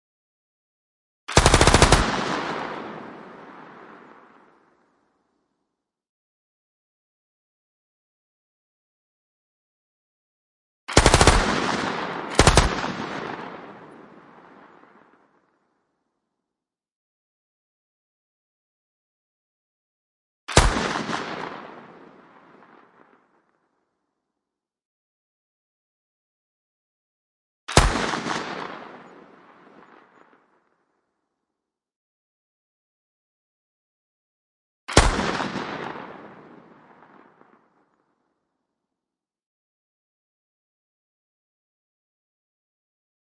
Light Machine Gun, Multiple Burst, Sound Effect
battle war zone fighting, small arms, and large caliber arms, great for combat, war, action video games, and back ground sound for videos. thank you for your support, if you need a specific sound contact us by email at: our home